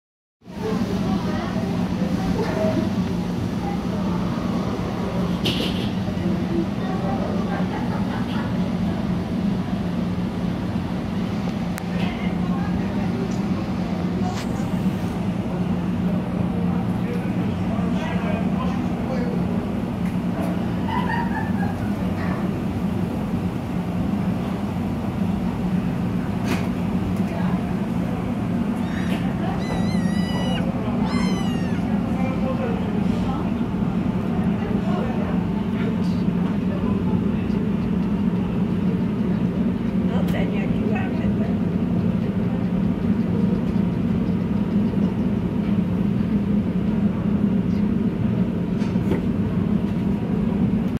Cute kittens playing, China
Two adorable kittens were playing in a street in Chinease old town of Pingyao.